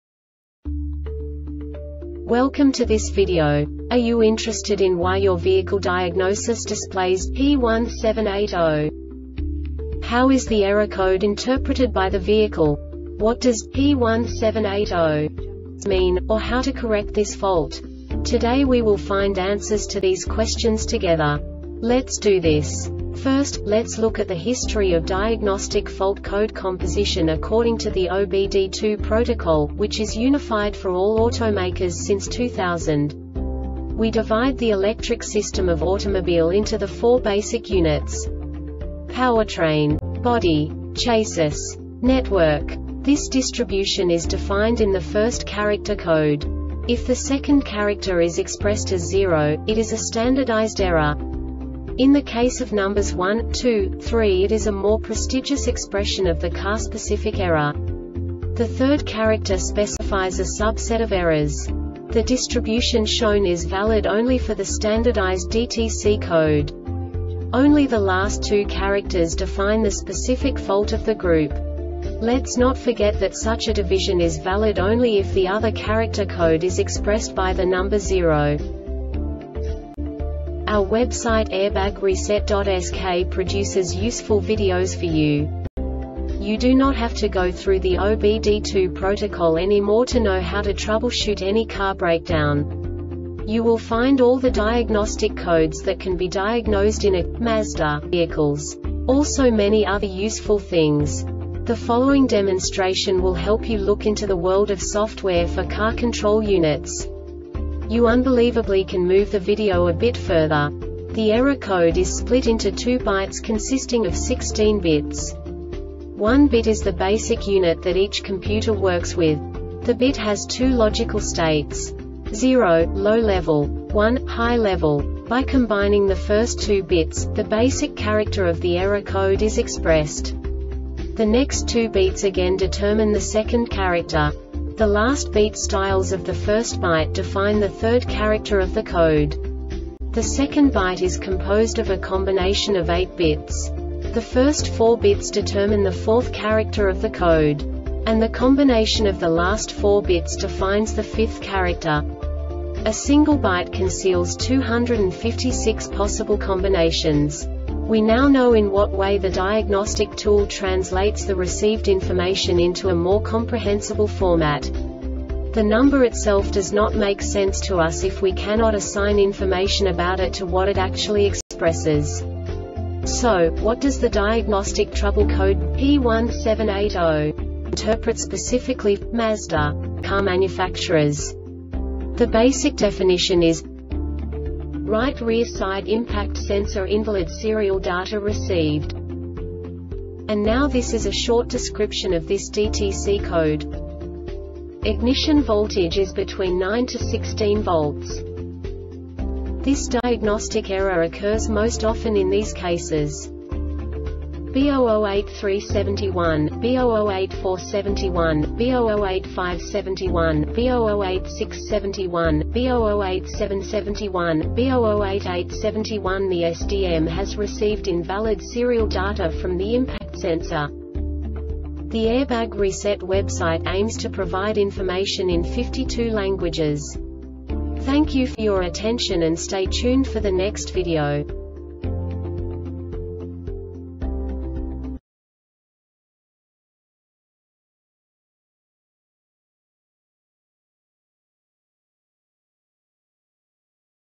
DTC Mazda P1780 Short Explanation
Contents:
0:21 Basic DTC analysis according to OBD2 protocol standard.
1:48 Insight into programming
2:58 A diagnostically understandable description of the Diagnostic Trouble Code
3:05 Basic error definition

Make:
Mazda
Code:
P1780
Definition:
Right Rear Side Impact Sensor Invalid Serial Data Received
Description:
Ignition voltage is between 9-16 volts.
Cause:
B0083 71, B0084 71, B0085 71, B0086 71, B0087 71, B0088 71The SDM has received invalid serial data from the impact sensor.